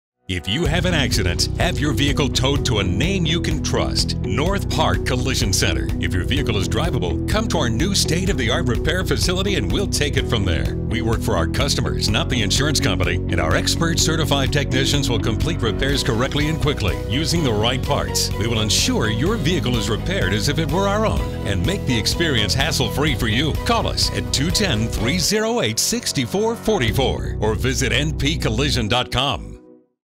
North Park Collision Center - Our New State-of-the-Art Facility
If you have an accident, have your vehicle towed to a name you can trust, North Park Collision Center. If your vehicle is drivable, come to our new state of the art repair facility and we’ll take it from there. We work for our customers, not the insurance company, and our expert certified technicians will complete repairs directly and quickly using the right parts. We will ensure your vehicle is repaired as if it were our own and make the experience hassle free for you.